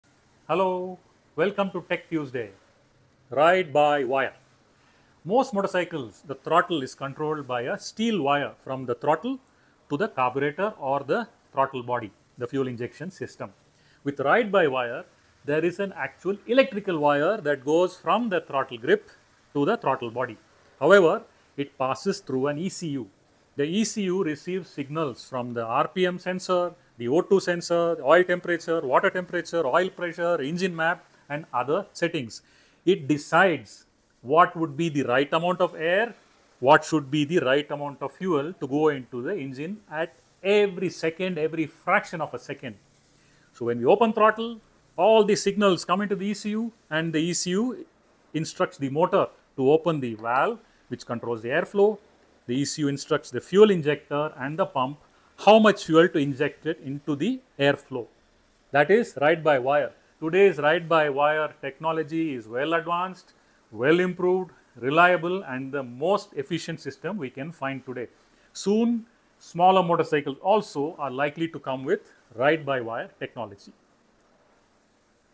Big Bear Tech Tuesday - What is "Ride by wire"?
What is Ride-by-wire? Here's a short explanation.